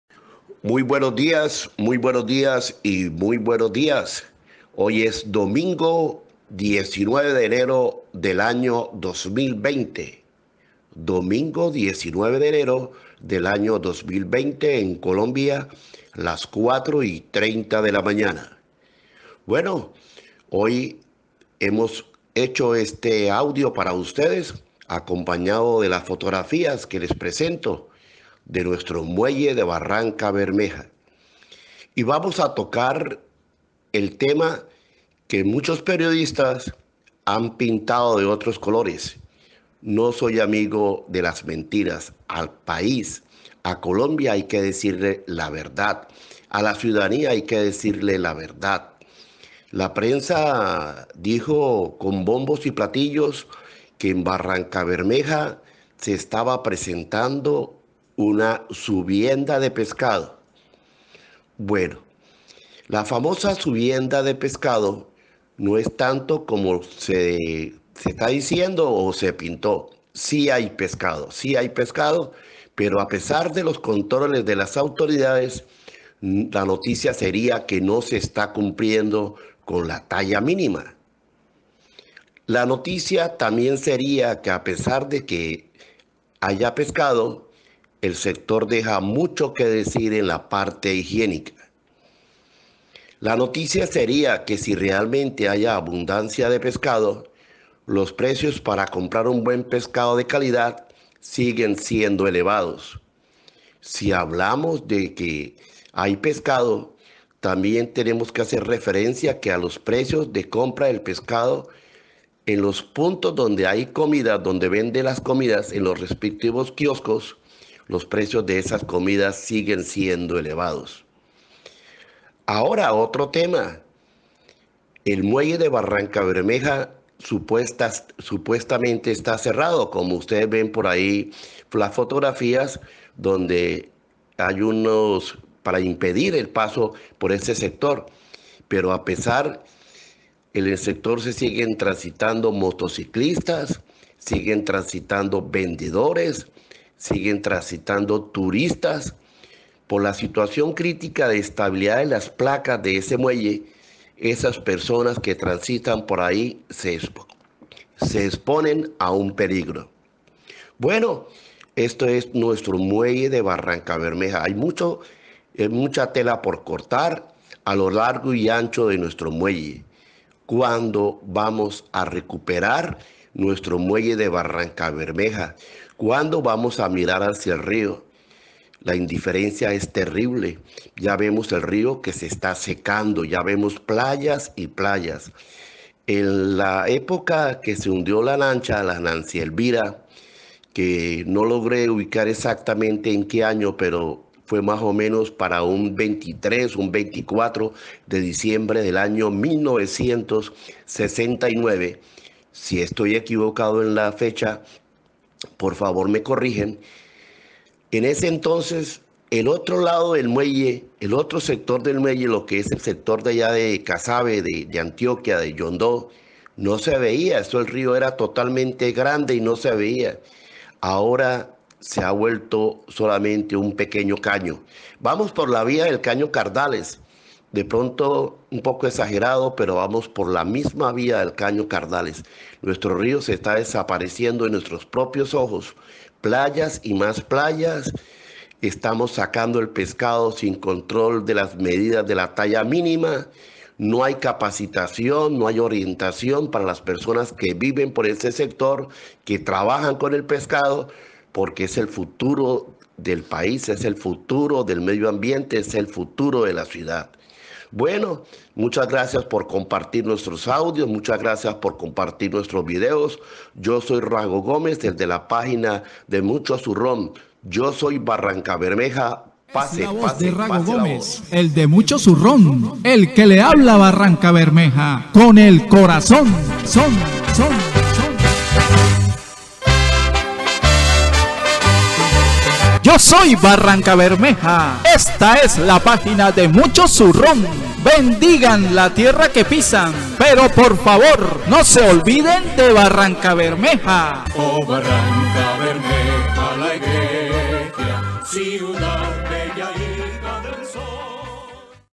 BARRANCABERMEJA.... MI VERDAD.
Hoy les he grabado un mensaje con mi voz, solo espero que mi comentario llega a oídos de las personas encargadas de manejar el futuro de nuestro municipio.
Ya es hora, ya es el momento indicado para volver a la disciplina y al orden que hemos perdido en las últimas administraciones de la ciudad.